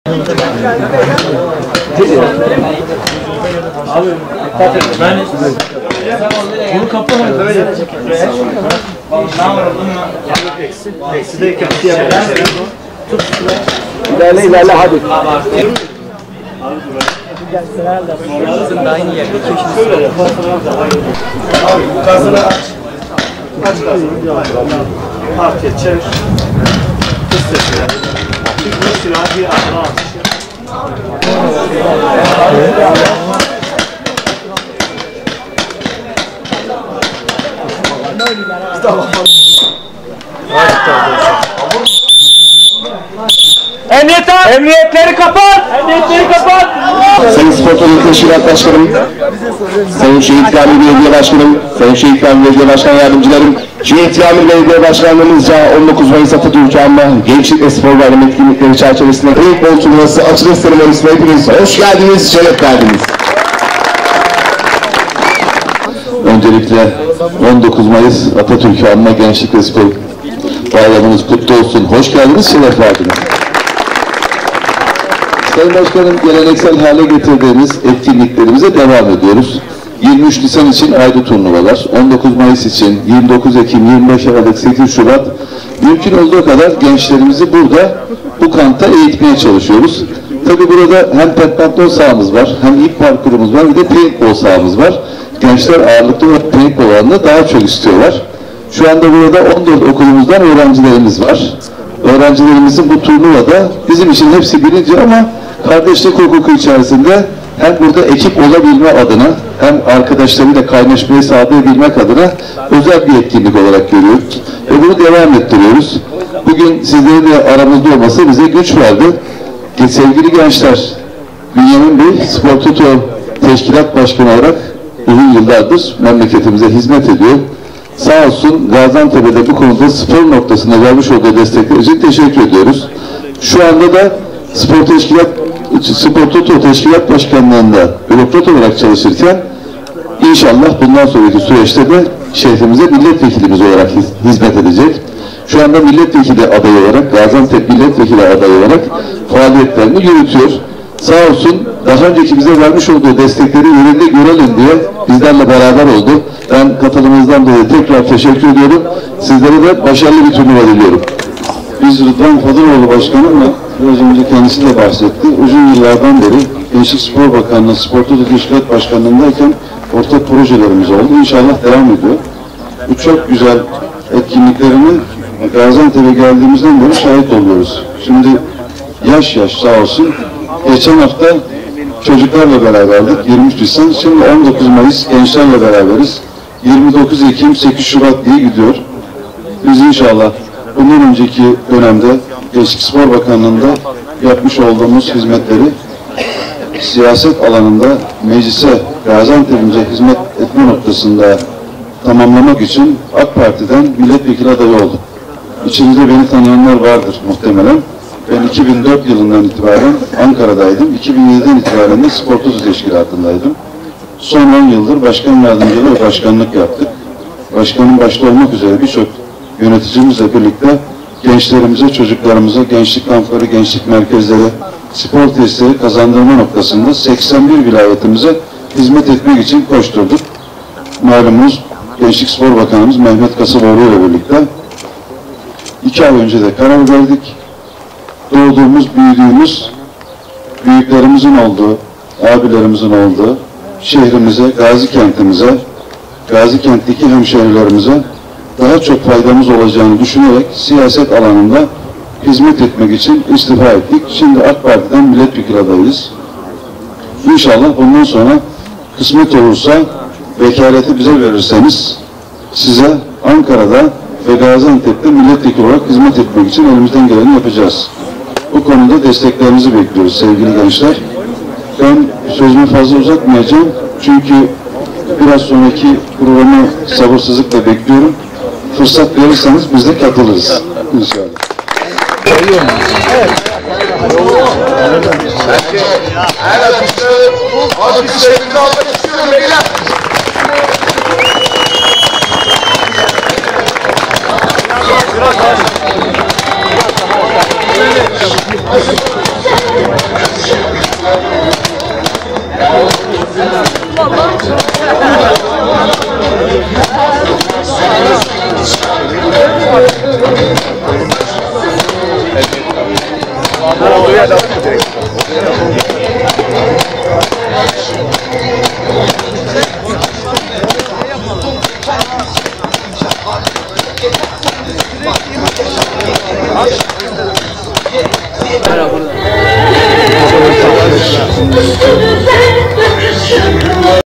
Fadıloğlu İle Bozgeyik, Turnuvayı Başlattılar
Şehitkamil Belediye Başkanı Rıdvan Fadıloğlu ile AK Parti Gaziantep Milletvekili Adayı Bünyamin Bozgeyik, 4. Geleneksel Paintball Gençlik Kupası Açılış Seremonisinde gençlerle bir araya gelerek, büyük ilgi gören organizasyonun startını veren atışları yaptılar.